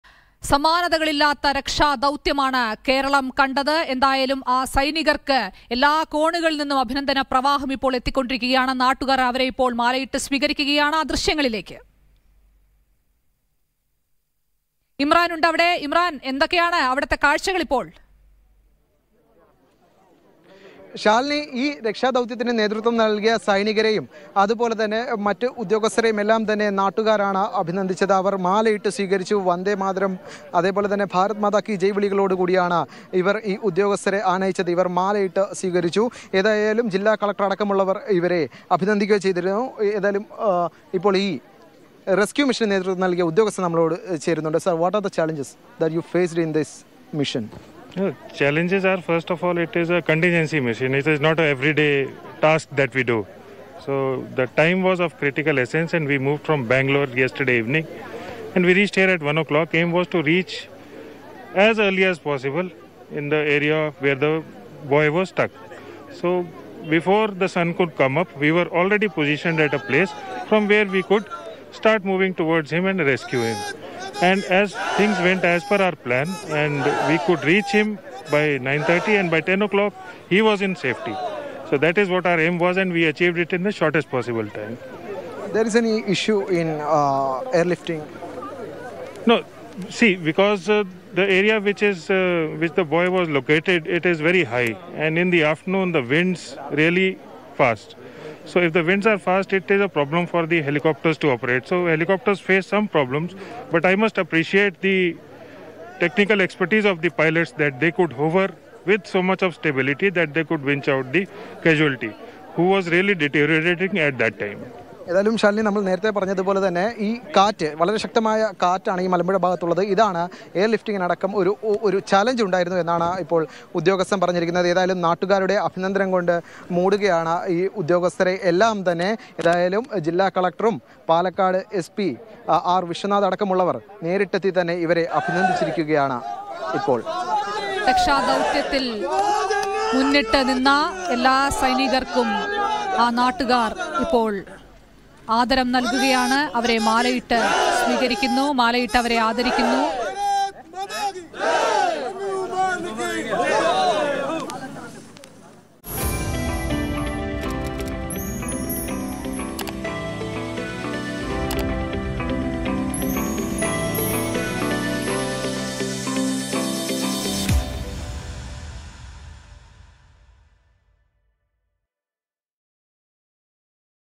പൂക്കൾ വിതറി, രക്ഷാപ്രവർത്തകരെ മാലയിട്ട് സ്വീകരിച്ച് നാട്ടുകാർ | Babu rescue Operation
പൂക്കൾ വിതറി, രക്ഷാപ്രവർത്തകരെ മാലയിട്ട് സ്വീകരിച്ച് നാട്ടുകാർ |